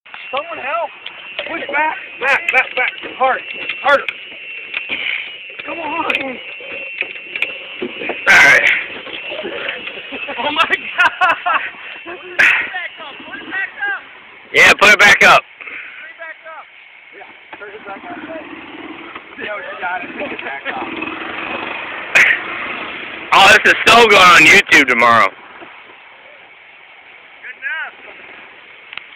Golf Cart stuck on tree.
In an attempt to run over a 14ft tall tree the golf cart got hung up a little. Thankfully the tree was able to be quickly repaired